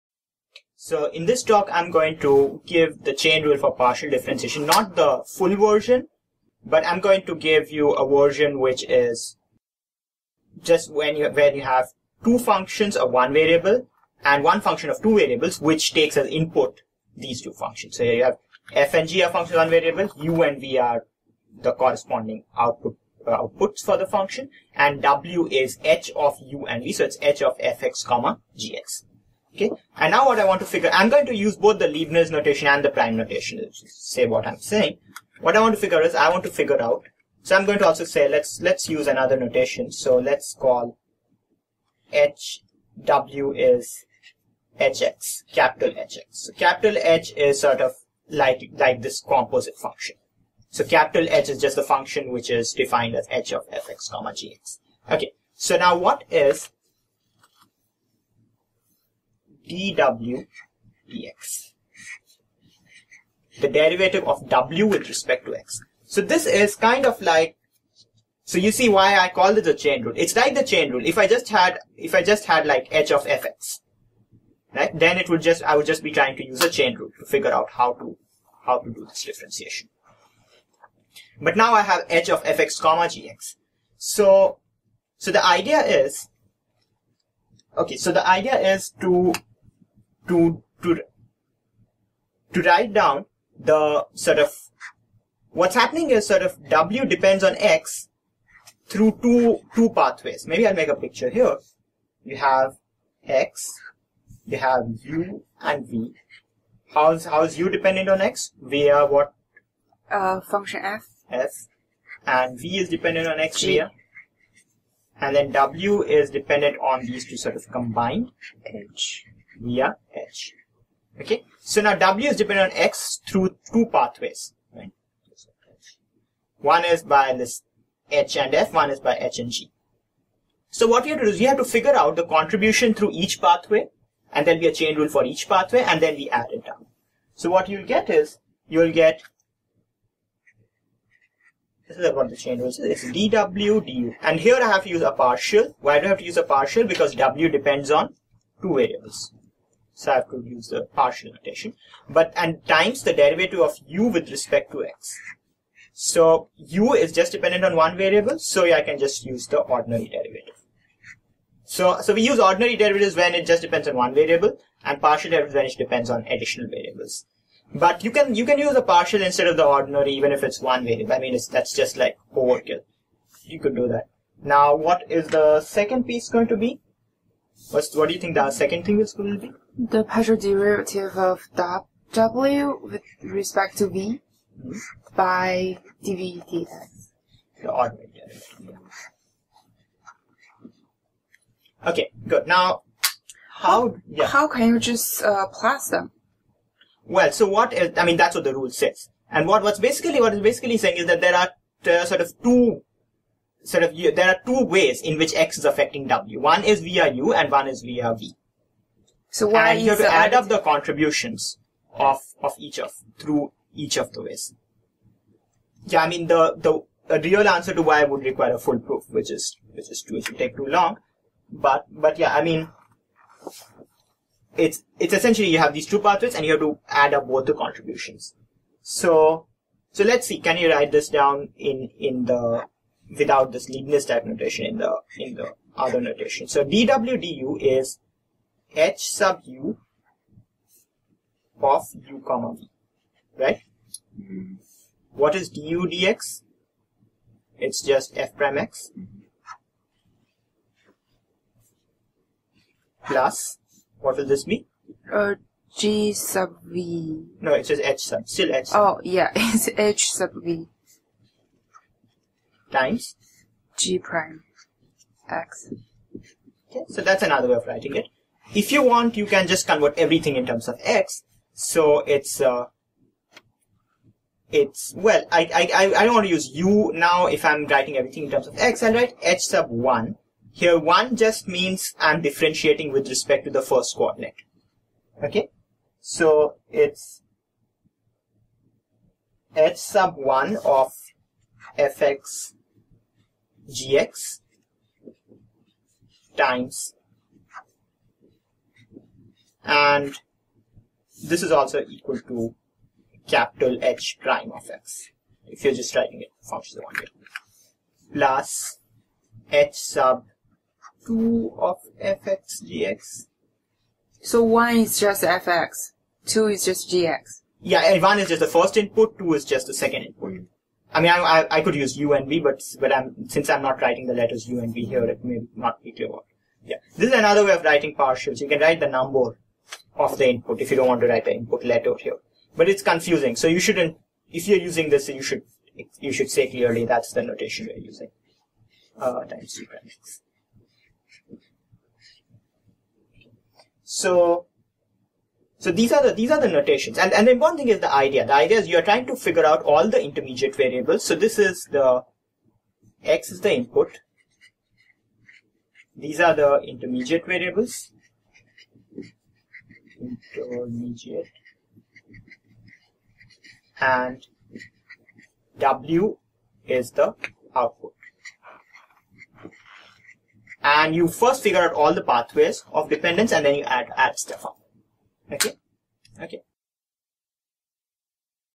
Chain rule for partial differentiation: simple case for beginners
Video created by Ruinan Liu and Vipul Naik.